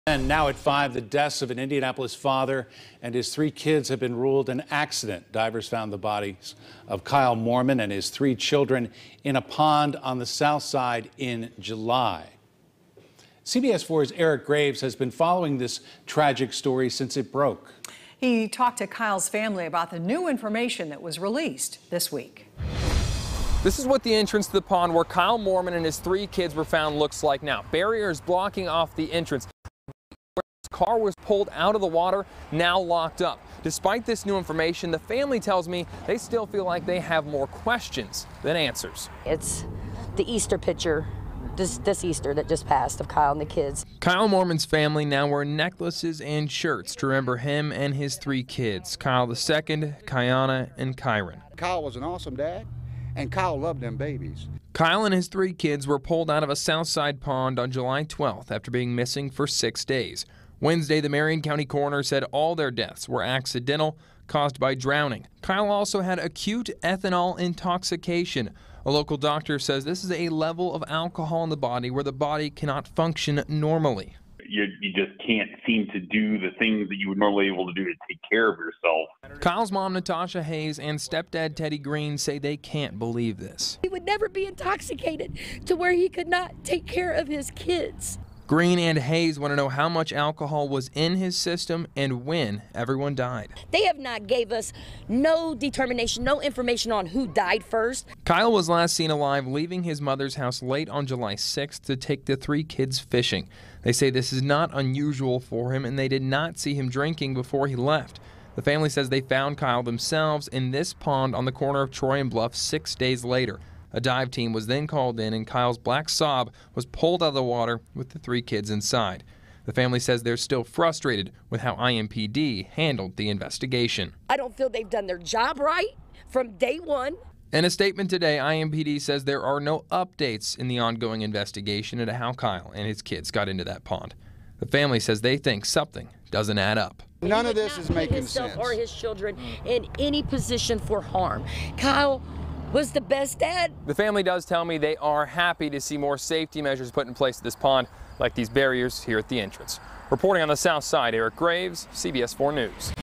Family of Kyle Moorman and kids react to news of accidental death
The deaths of an Indianapolis father and his three kids have been ruled an accident by the Marion County Coroner.